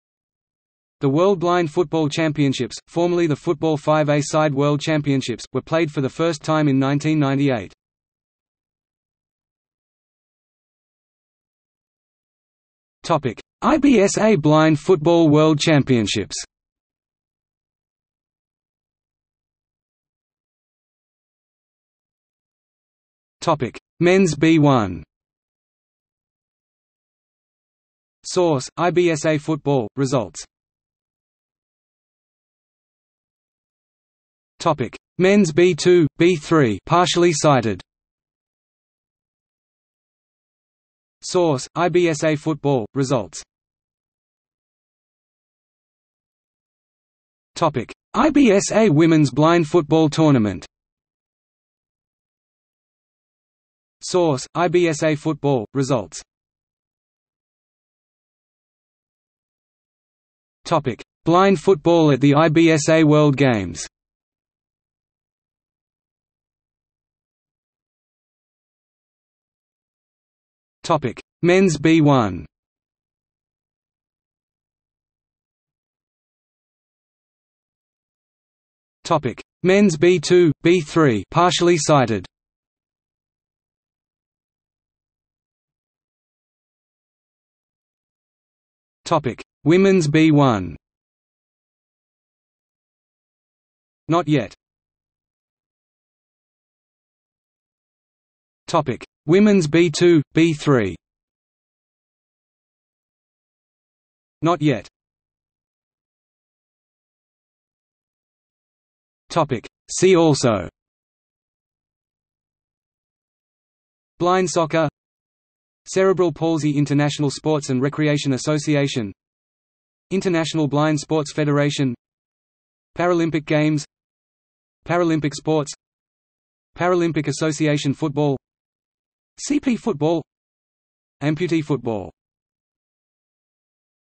World Blind Football Championships | Wikipedia audio article
00:00:09 1 IBSA Blind Football World Championships
00:00:20 1.1 Men's B1
00:00:31 1.2 Men's B2/B3 (Partially Sighted)
00:00:44 1.3 IBSA Women's Blind Football Tournament
00:00:57 2 Blind Football at the IBSA World Games
00:01:07 2.1 Men's B1
00:01:15 2.2 Men's B2/B3 (Partially Sighted)
00:01:25 2.3 Women's B1
00:01:35 2.4 Women's B2/B3
00:01:45 3 See also

- Socrates

SUMMARY
The World Blind Football Championships, formerly the Football-5-a-Side World Championships, were played for the first time in 1998.